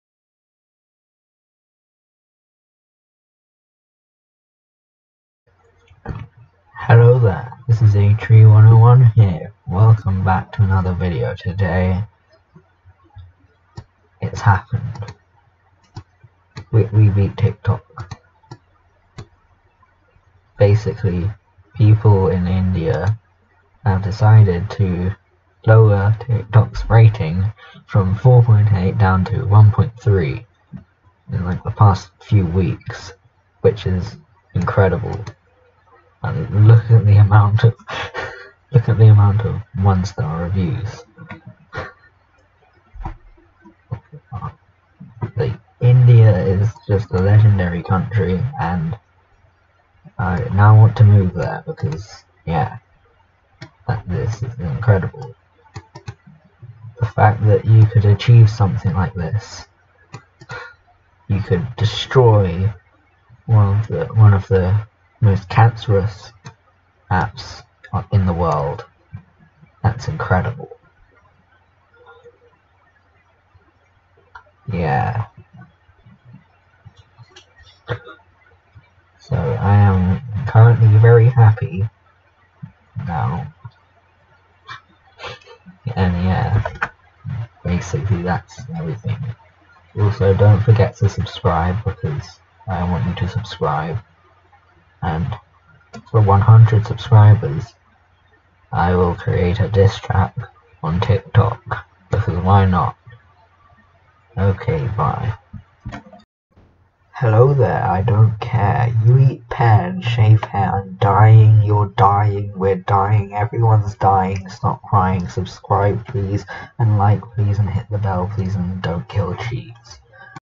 (old) Tiktok's ratings drop from 4.8 to 1.3 ...
In the past few weeks, India has lowered tiktok's ratings from 4.8 to 1.3. This has cured my depression.